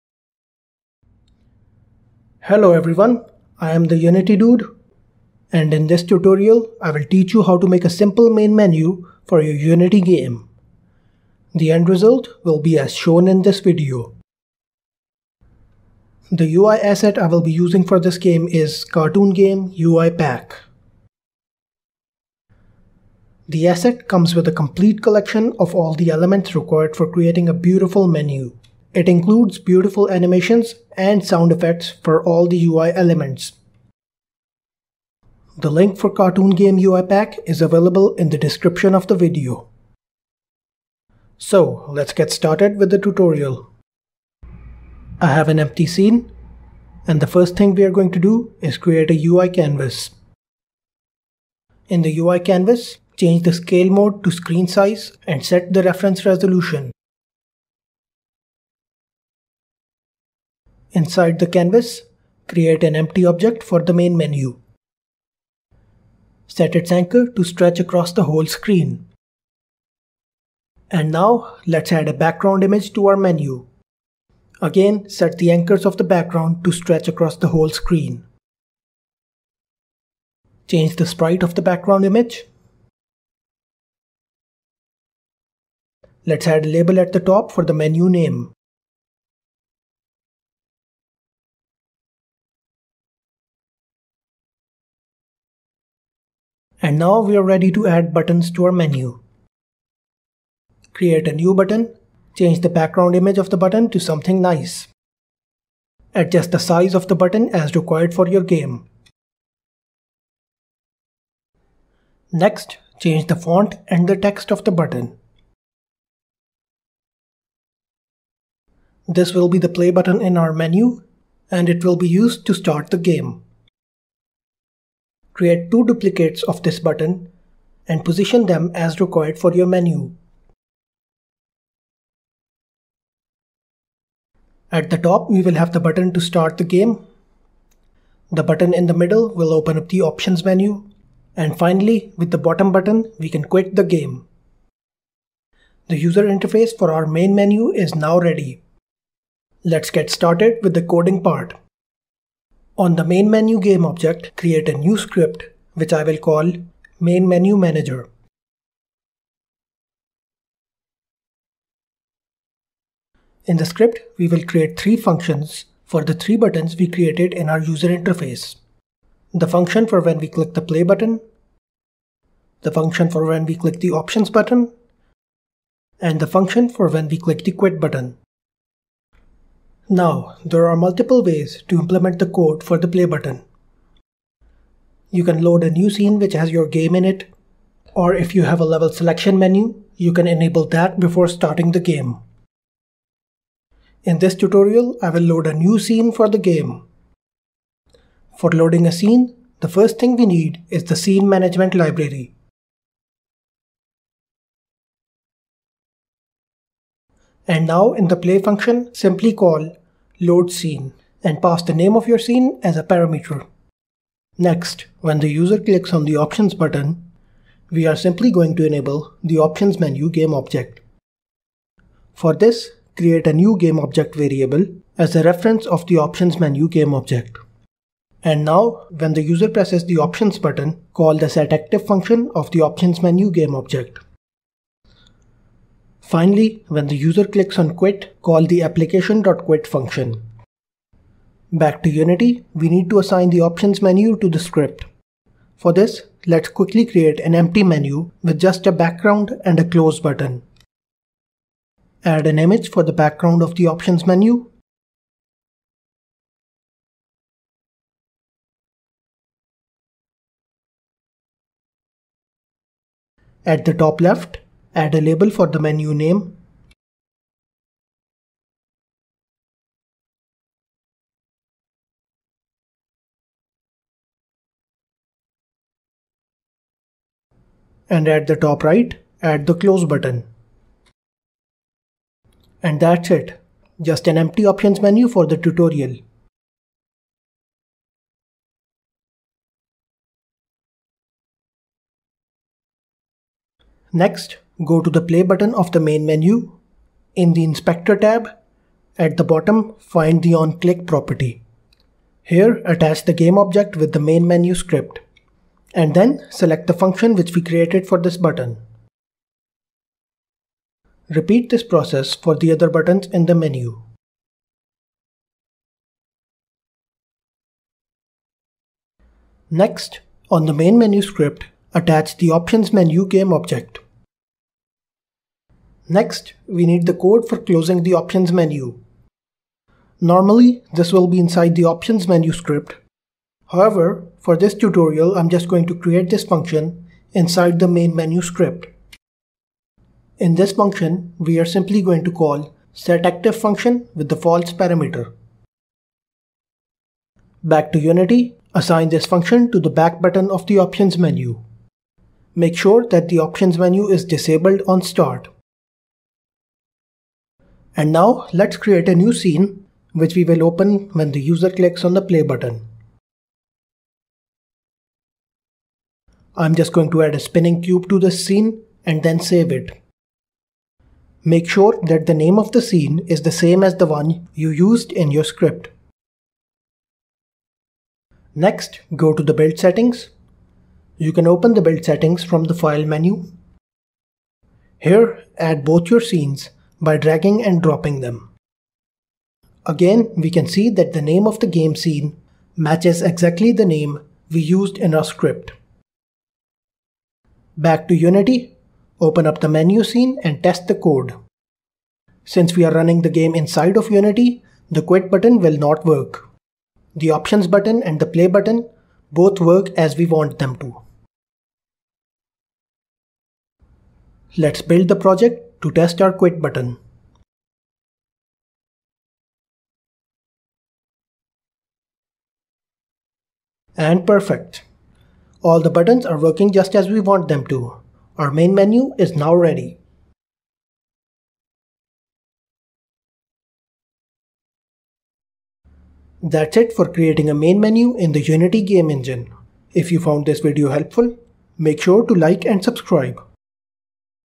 Build a MAIN MENU Quick and Easy | Unity Tutorial For Beginners
Main Menus are extremely important and are the first thing the user sees after opening your game.
In this Unity tutorial we'll quickly make a simple Main Menu.
We will implement three buttons : the Play button, the Options button and the Quit button.